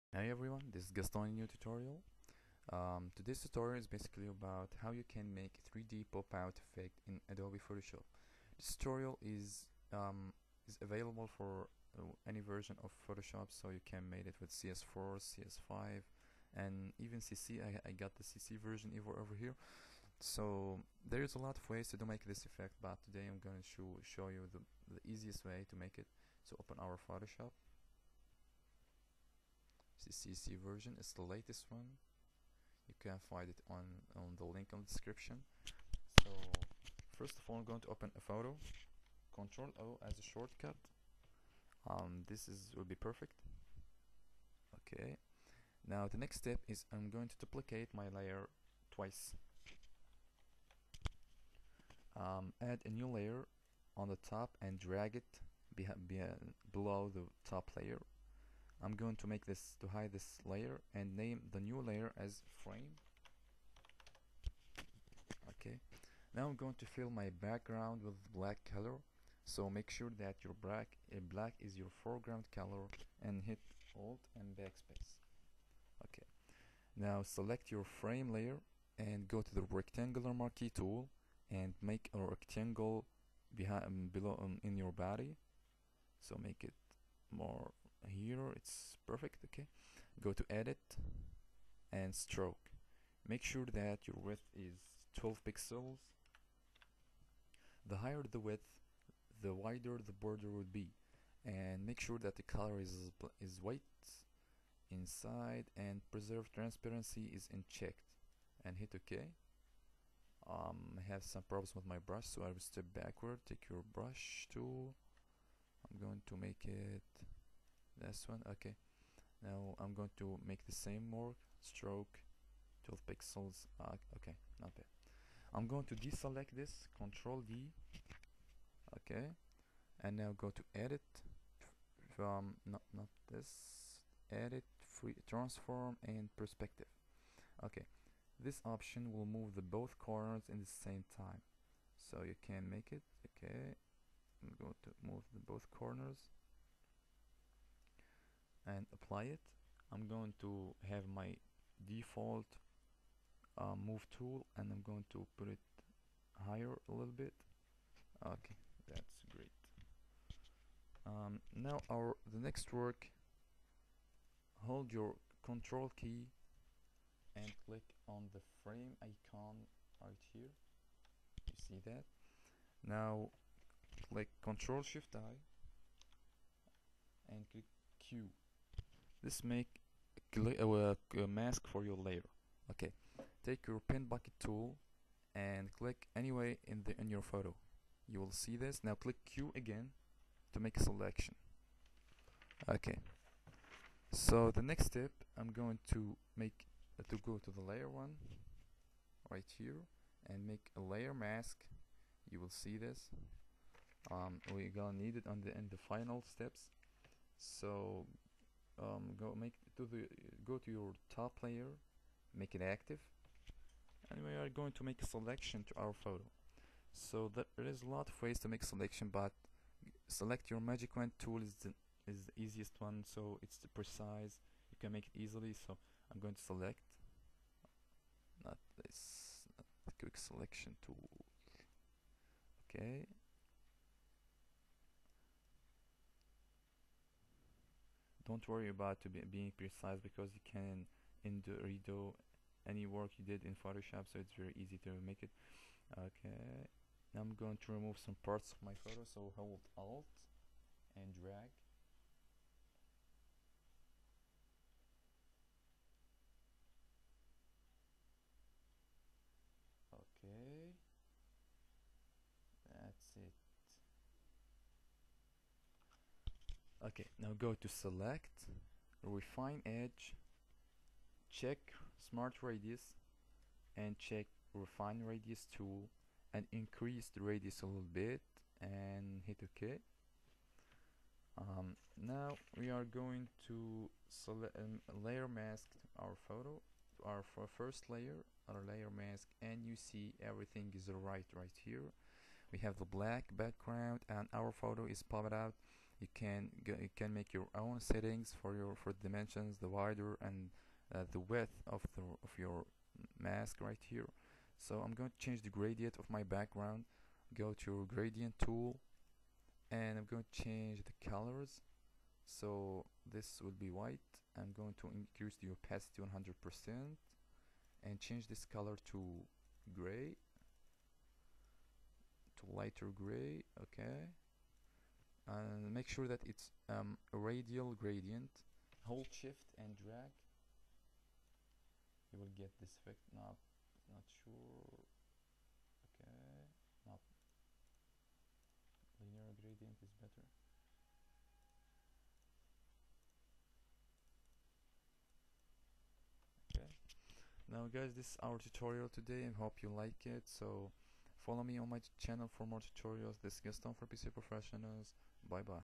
Photoshop How to Make a 3D,Pop-Out Photo Effect
Photoshop tutorial showing how to make a 3D, pop-out photo effect by making an object, animal or person look as if it's popping out from a photo.

- Video Made by Gaston Geek .

- Skype : gaston.geek